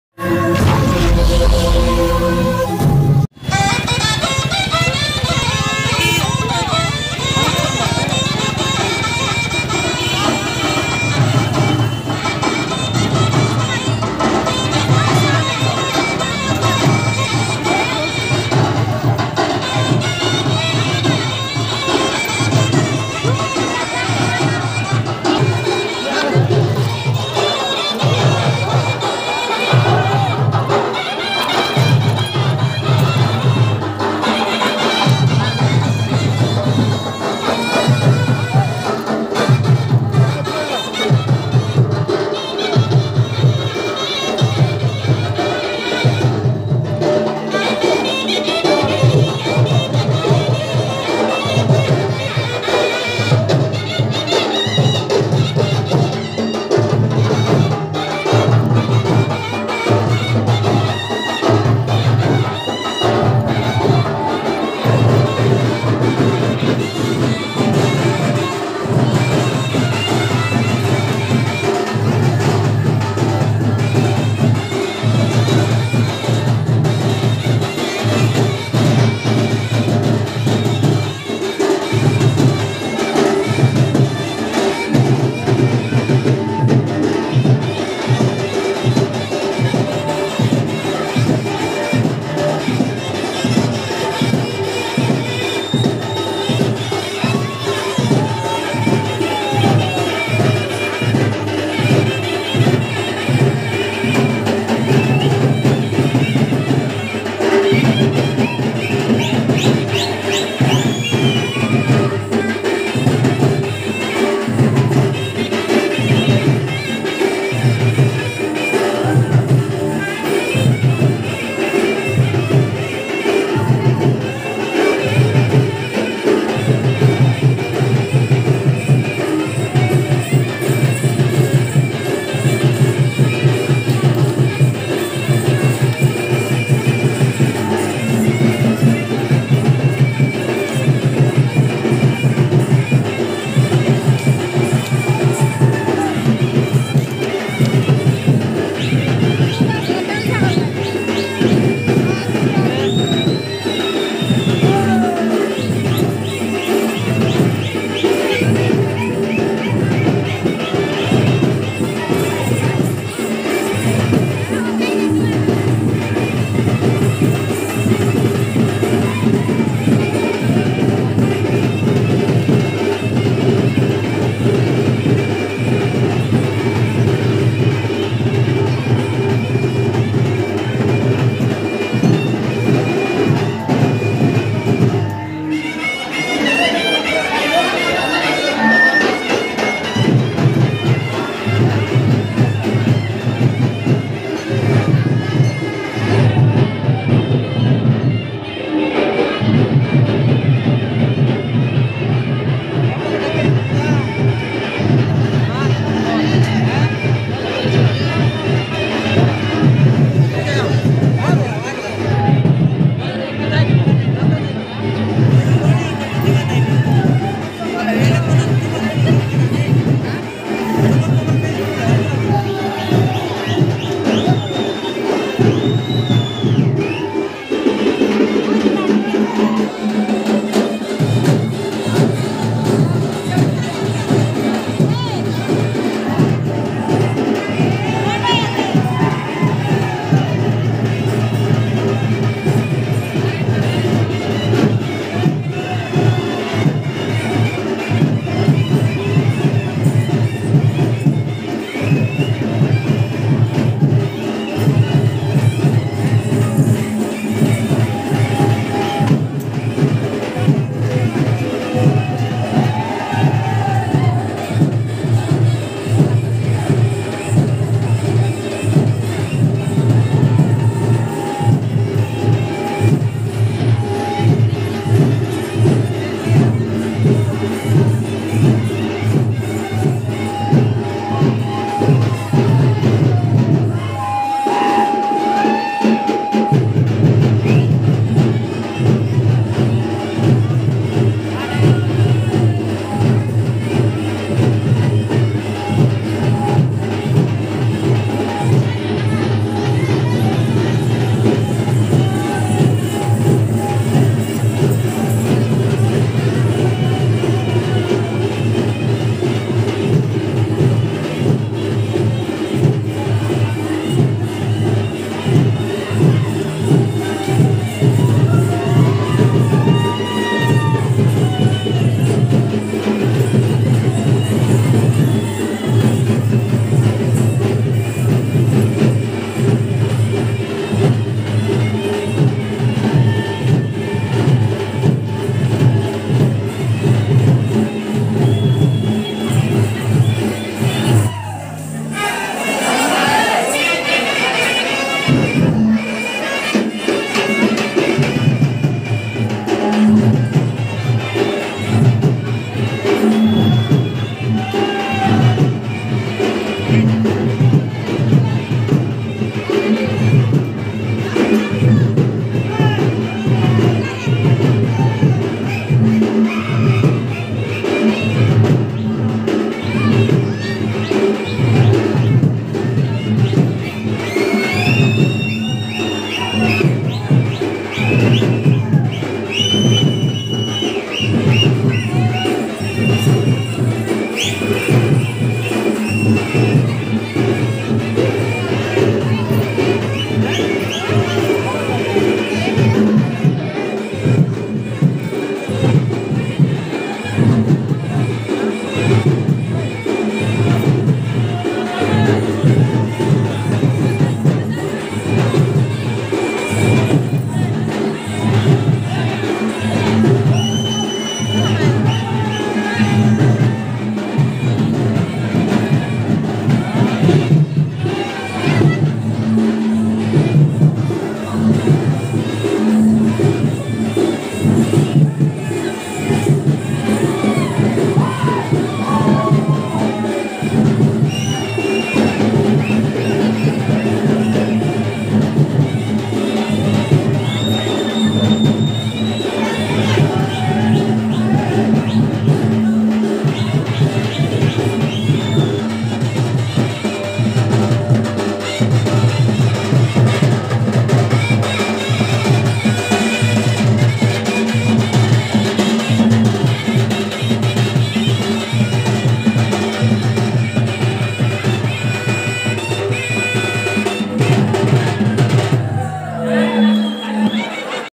Balda cave Dhemsa //Maa nageswary yatra Balda //Biggest mountain yatra
Hi

Dosto is video me maine aapke liye kuch hamara dist ka ek famous place and yatra maa nageswary caves yatra Balda ka  video upload kiya, Ummed hai ki aapko  is ka ek video pasand aayegi

sjsekhar4U# Bend baja dance # koraputia nato#

Dosto is channel pe aapke entertainment ke liye ek  famous place and mountain yatra maa nageswary caves yatra place balda ka video dhala.